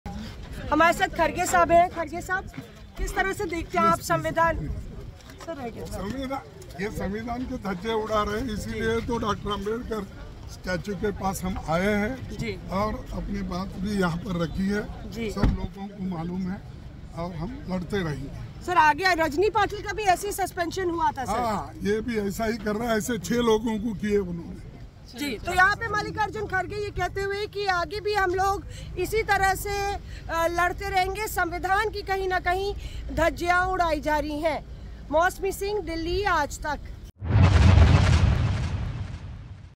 ‘ये संविधान की धज्जियां उड़ा रहे’ बोले Congress अध्यक्ष Mallikarjun Kharge | Parliament | Aaj Tak
राज्यसभा में मल्लिकार्र्जुन खड़गे बोले ऐसा पहली बार हो रहा है कि सदन में छोटी-छोटी बात पर कार्रवाई हो रही है. ये संविधान की धज्जियां उड़ा रहे.